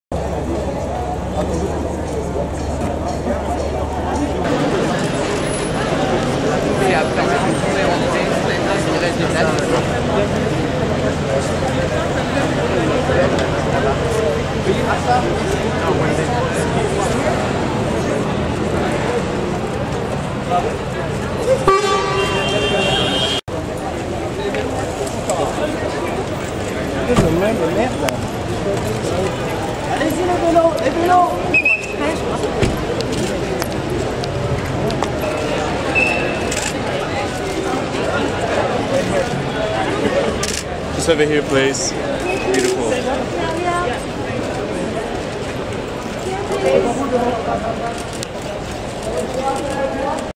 Jeremy O’Harris (Slave play, Emily in Paris) etc..@ Paris 4 july 2022 Fashion Week show Schiaparelli
Jeremy O’Harris (Slave play, Emily in Paris) and more guests @ Paris 4 july 2022 Fashion Week show Schiaparelli